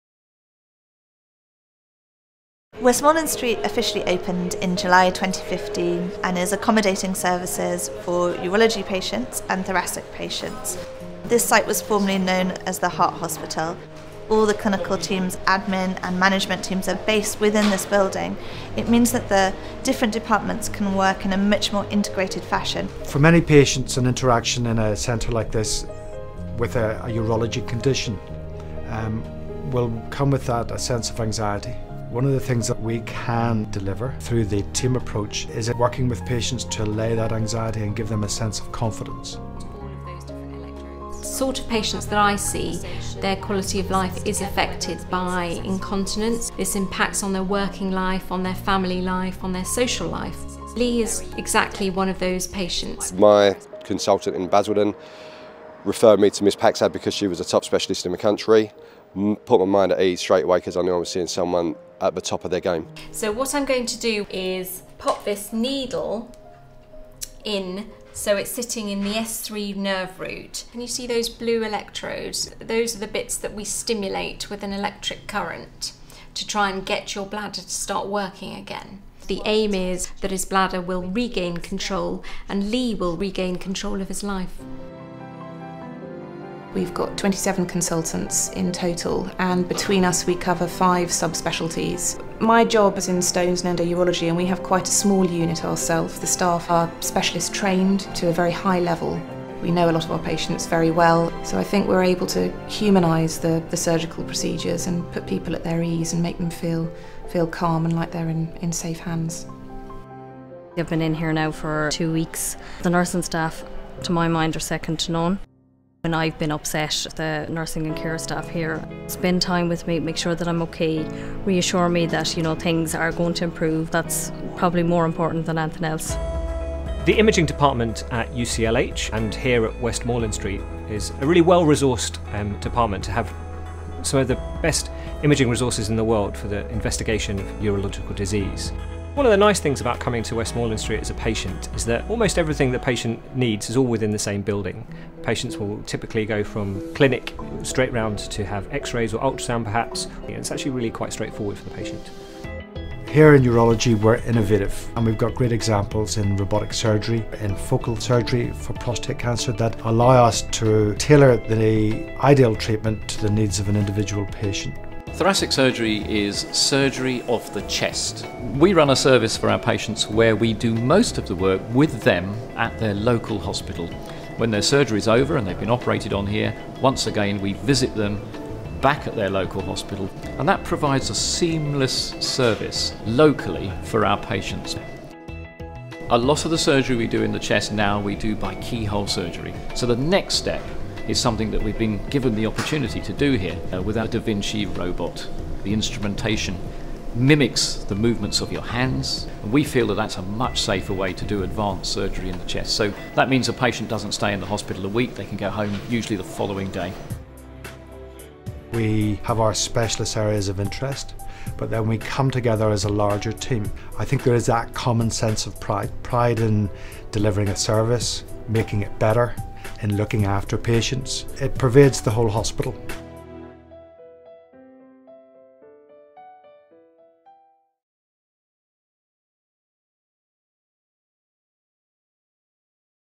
University College Hospital at Westmoreland Street
An introduction to the urology and thoracic services at University College Hospital at Westmoreland Street